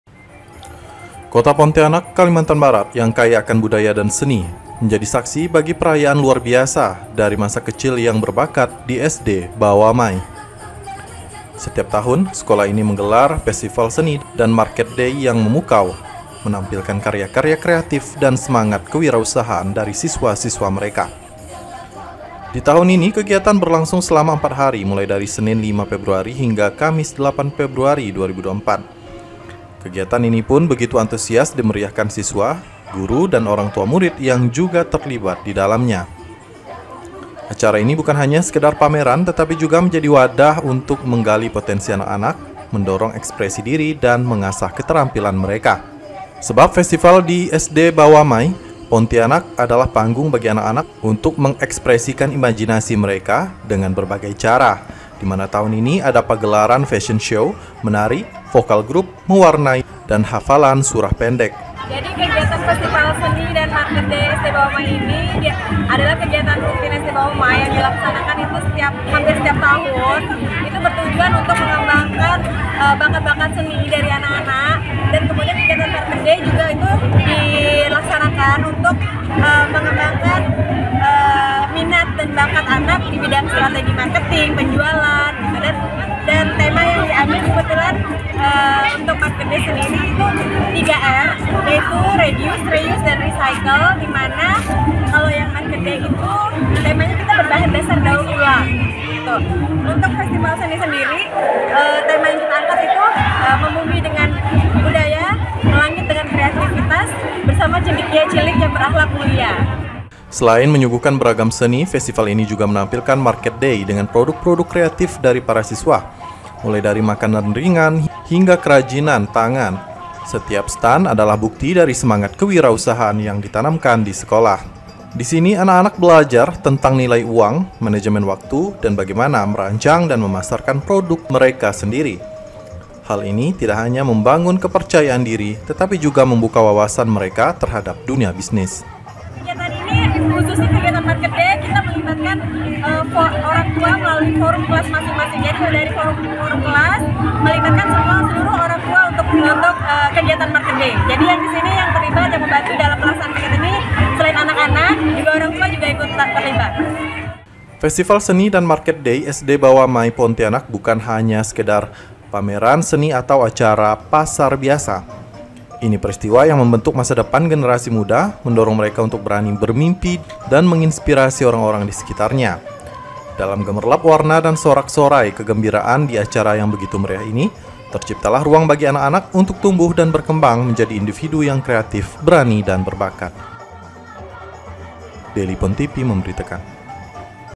Menggali Kreativitas Anak di Festival Seni dan Market Day SD Bawamai
Seperti tahun sebelumnya, SD Bawamai Pontianak kembali menggelar festival seni dan market day dengan menampilkan karya kreatif dan semangat kewirausahaan siswa.

Kegiatan dimulai Senin 5 Februari 2024 hingga Kamis mendatang ini, begitu antusias dimeriahkan siswa, guru dan orangtua murid, yang juga terlibat didalamnya.